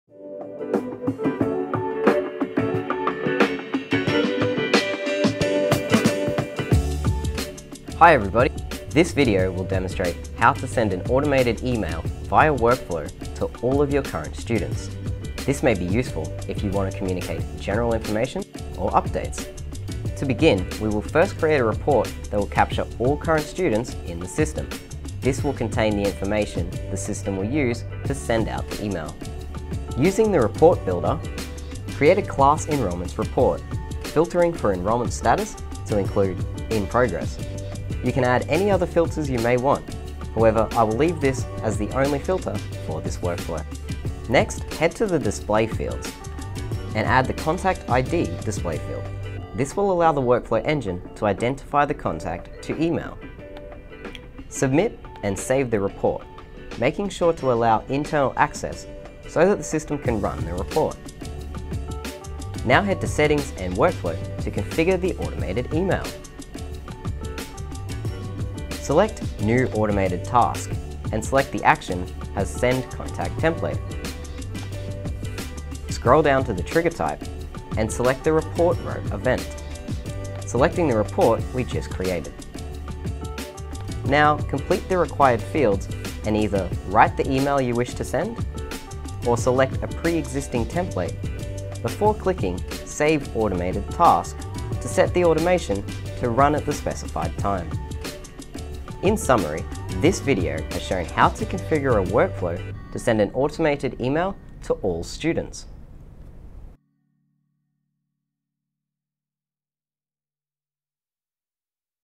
Workflow Series Ep.2 - Bulk Automated Email
This video will explain how to use the Workflow Engine to send an automated email on mass to all contacts within a particular parameter.
The example used in this video is to send a quick email reminder to all current students to wash their hands to stop the spread of the Coronavirus